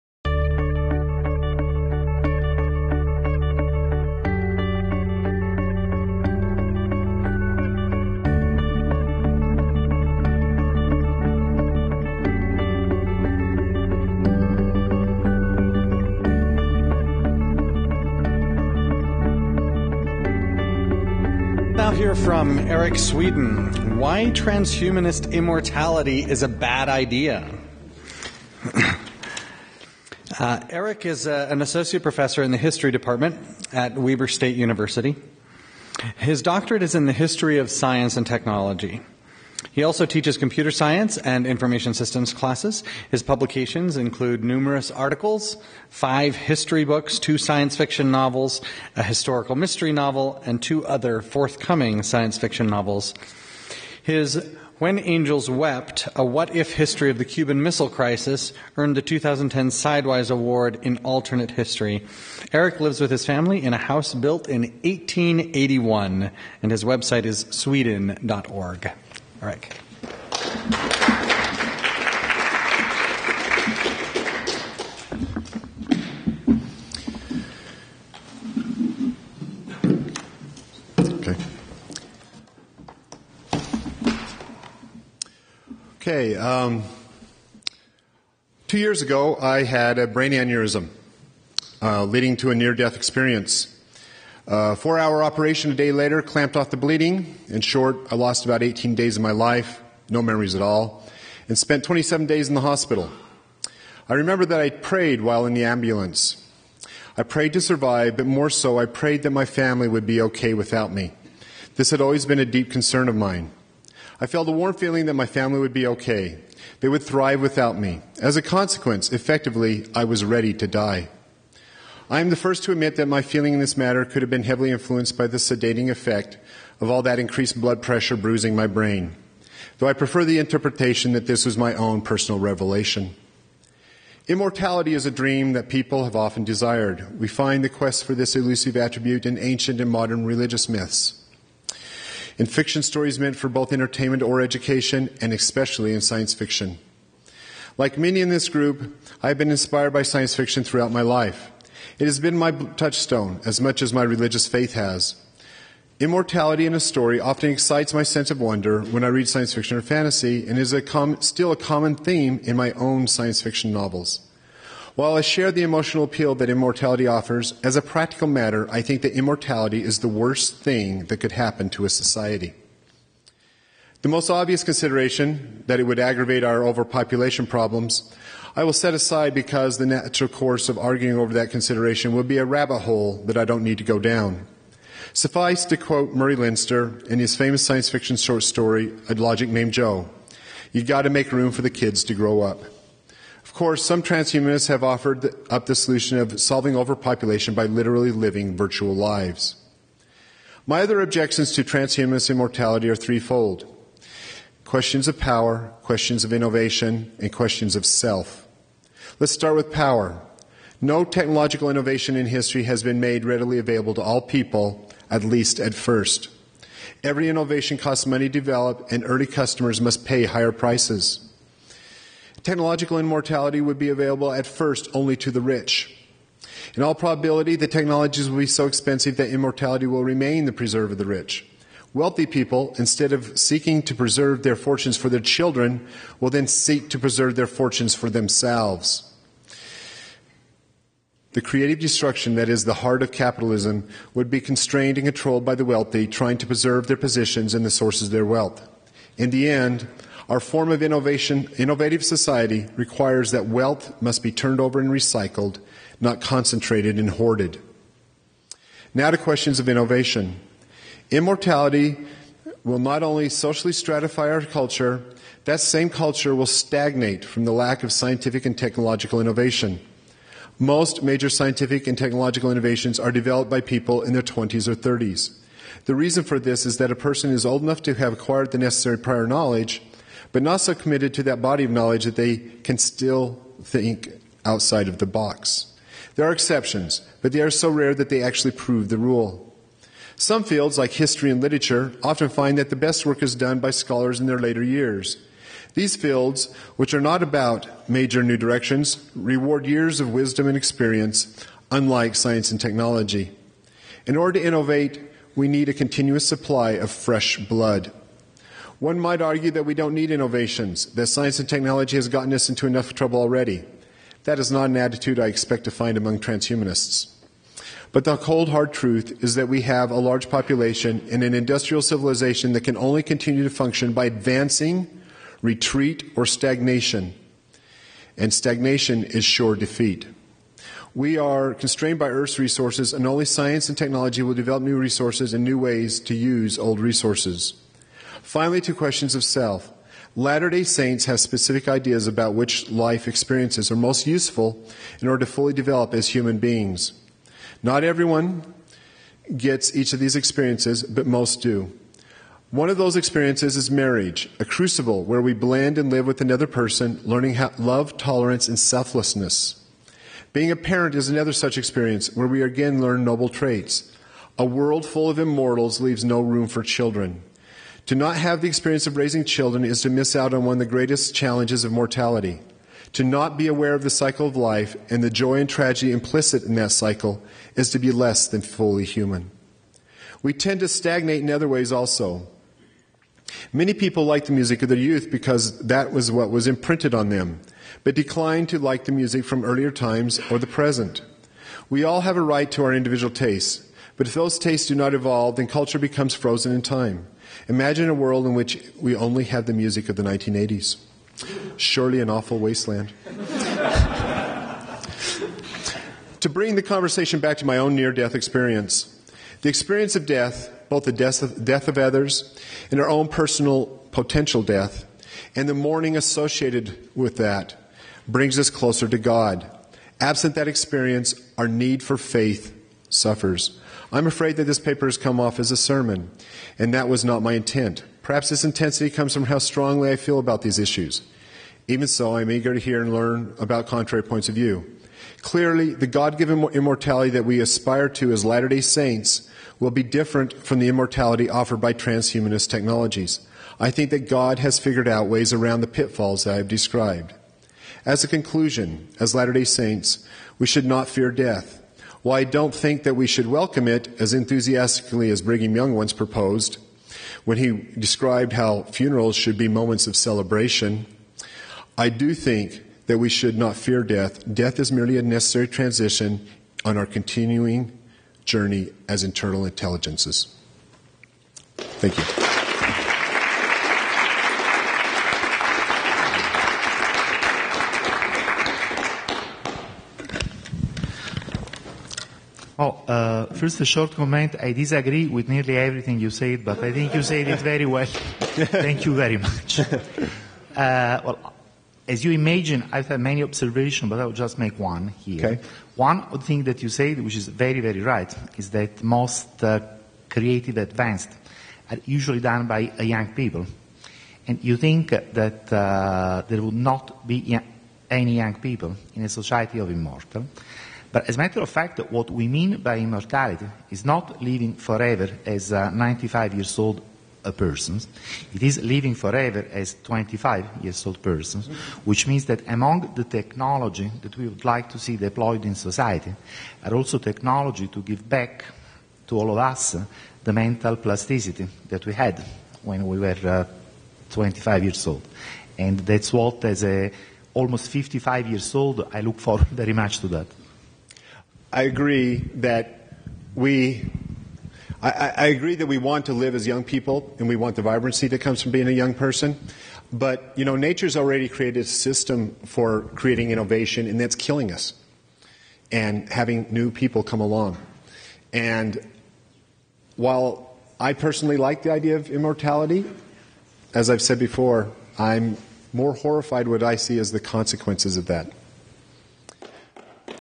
"Why Transhumanist Immortality is a Bad Idea" by Eric Swedin
The 2012 conference of the Mormon Transhumanist Association was held on 6 April 2012 from 9am to 5pm at the Salt Lake City Public Library in Salt Lake City, Utah. The conference is open to the public.